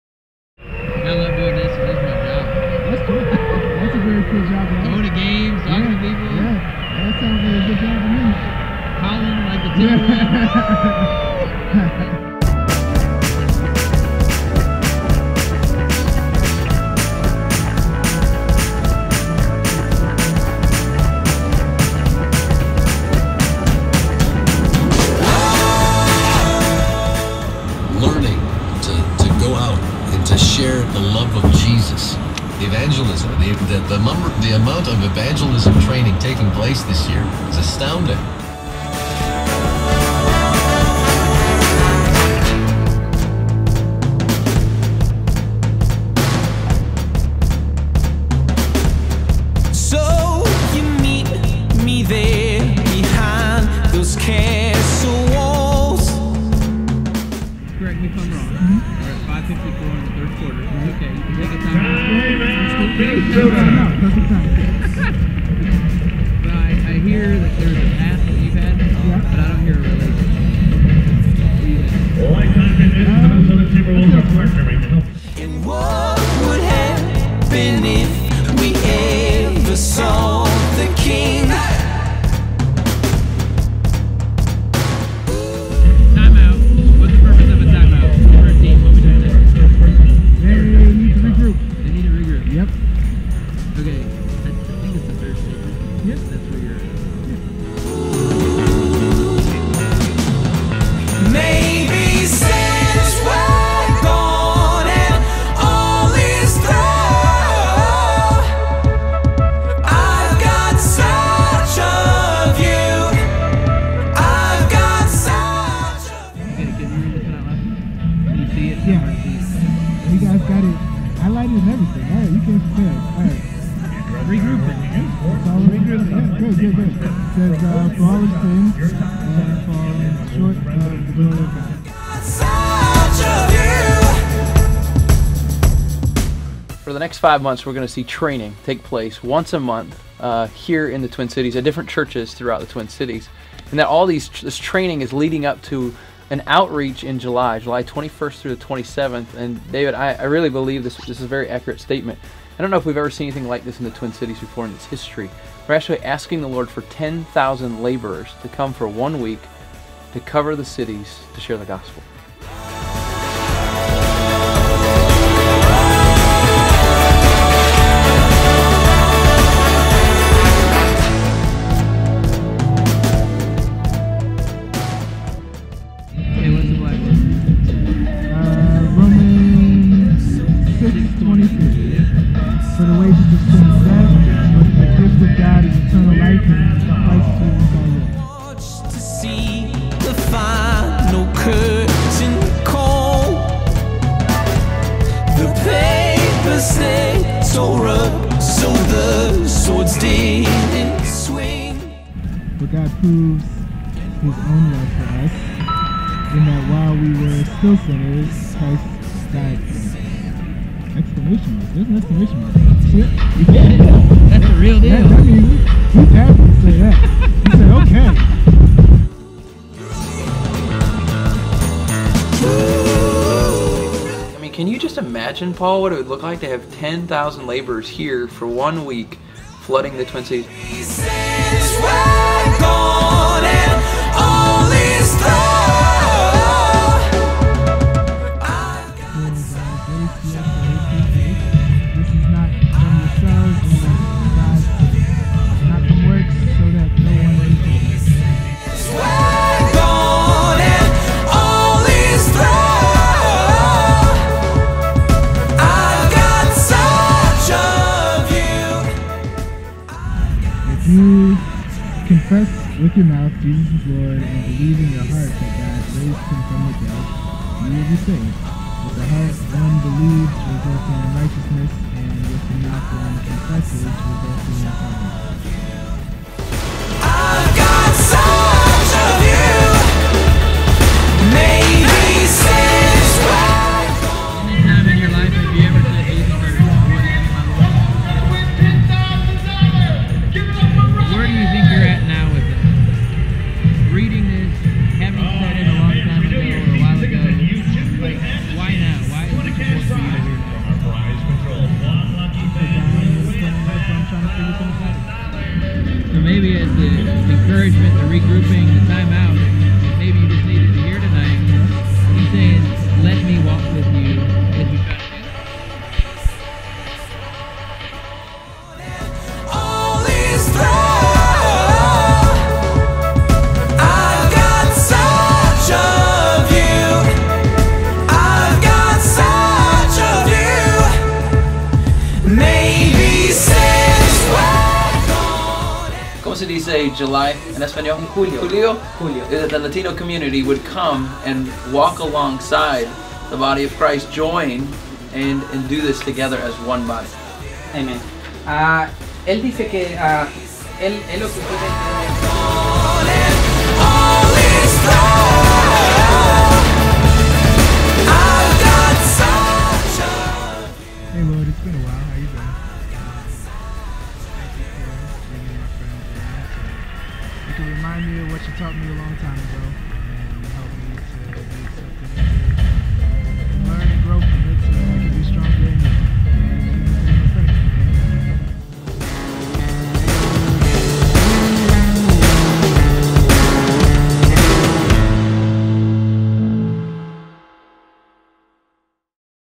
reviveTWINCITIES :: Prep Trip #5 :: Day 2
~ ~ Kyle Lance Martin just announced our 10th city - Twin Cities and now he and the team from TTR are prepping for our Engage Trip on July 21 - 27th. Come join us! FB reviveTWINCITIESmn or Twitter @reviveTWINCITYS ~ ~